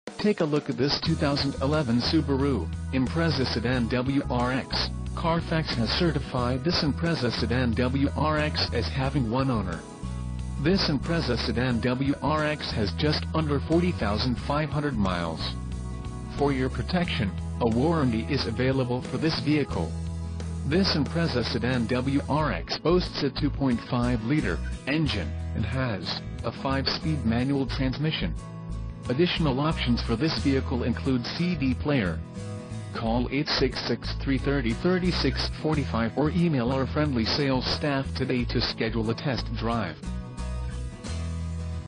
2011 Subaru Impreza Sedan WRX 4dr Manual WRX Sedan - Rolling Meadows, IL
Exotic Motors, Rolling Meadows, IL - 2011 SUBARU IMPREZA WRX AWD SEDAN!! 5-SPEED CD-PLAYER 17"ALLOYS 1-OWNER!!